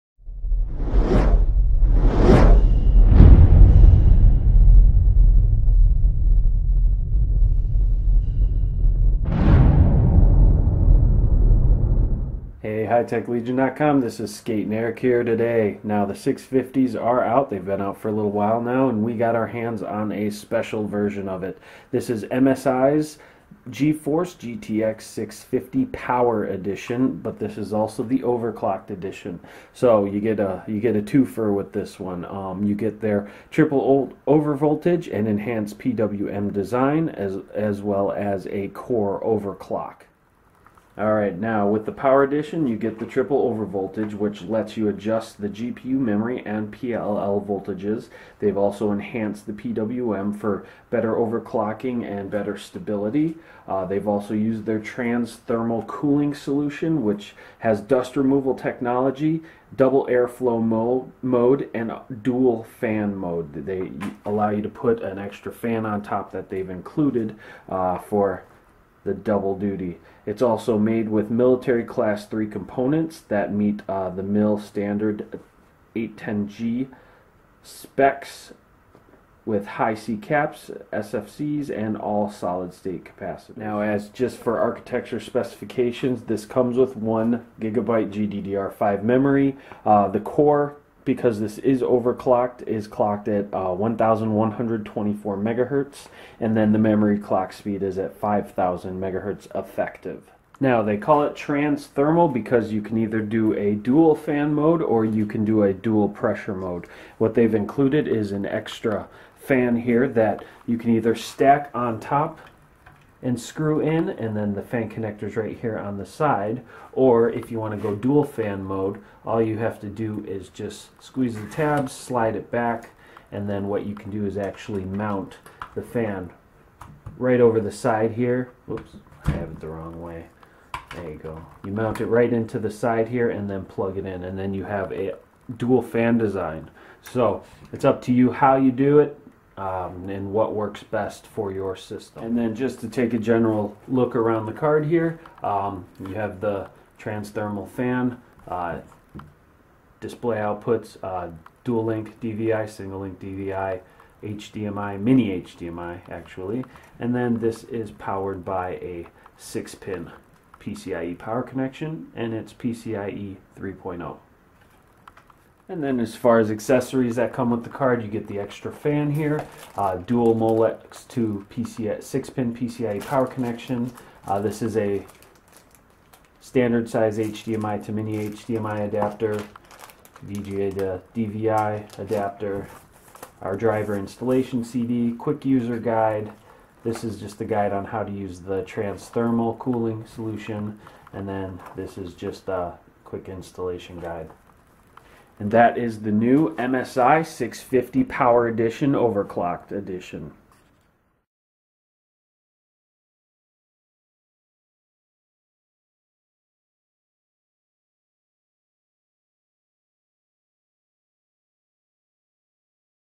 MSI Geforce GTX 650 Power Edition Overclocked Video Card Unboxing and Overview
Great deals on this and other Video cards

Featuring a core clock speed of 1124MHz (66 MHz higher than stock) MSI has released the GeForce GTX 650 Power Edition OC. The MSI GeForce GTX 650 Power edition has 1024MB of GDDR5 memory clocked at 5000MHz and runs on a 128-bit bus. It is targeted at the budget gamer and is designed to offer 1080p gaming to all, while using energy efficiently. MSI has produced the GPU using military class components that meet the MIL-STD-810G standard for increased durability and longevity. The GTX 650 Power Edition includes the ability to performance tune the card further using MSI's own Afterburner utility. Connectivity is provided through a mini-HDMI port, DVI-D, or DVI-I port allowing the GPU to be connected to just about any display on the market.